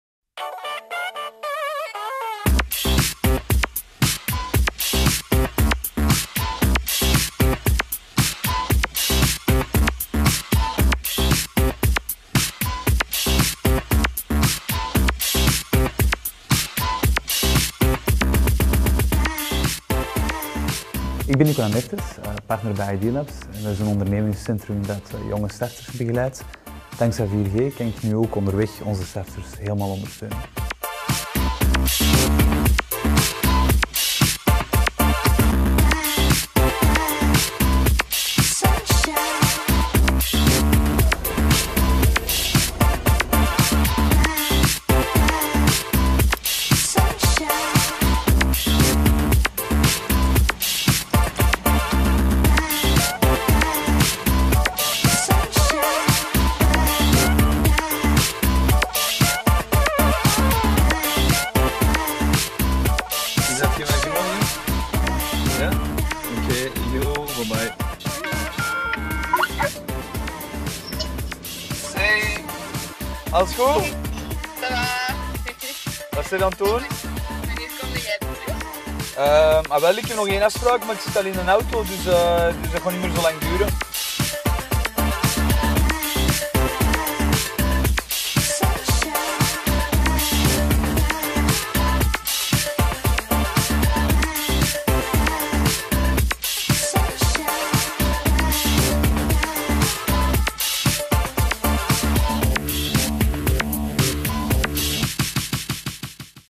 Idealabs partner Nicolas Mertens is altijd connected via Mobistar 4G.
Ook onderweg kan Idealabs partner Nicolas Mertens verder blijven werken dankzij het 4G-netwerk van Mobistar. Ideaal om altijd in contact te blijven met de jonge starters die hij begeleidt.

Alle producten, logo's , merken die vermeld of getoond worden in deze video zijn ongewild en toevallig opgenomen op het publieke domein.